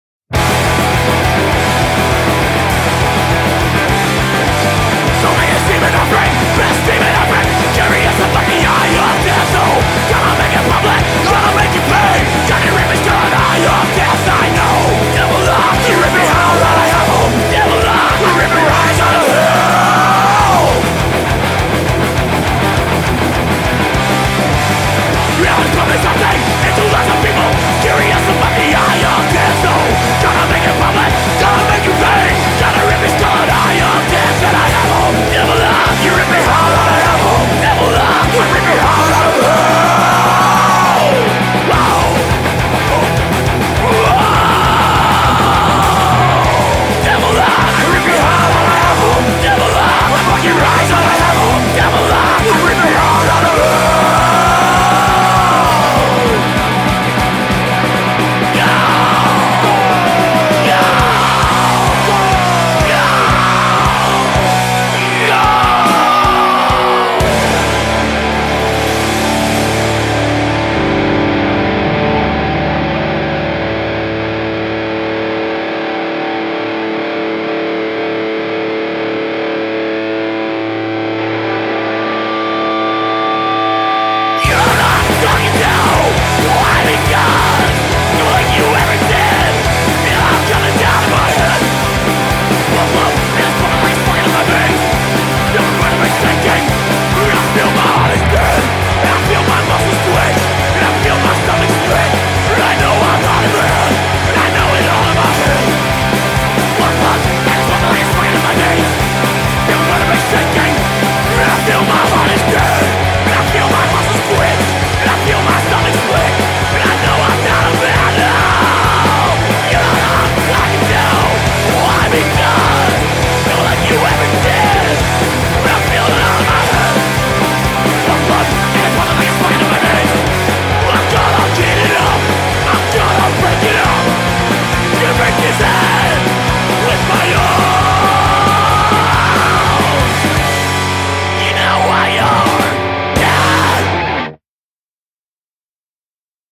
Hellmouth Misfits Tribute
Songs off the giveaway 7" from the second American Hardcore Tribute show (Misfits & Dead Kennedys) with Child Bite, Old Gods and Golden Torso.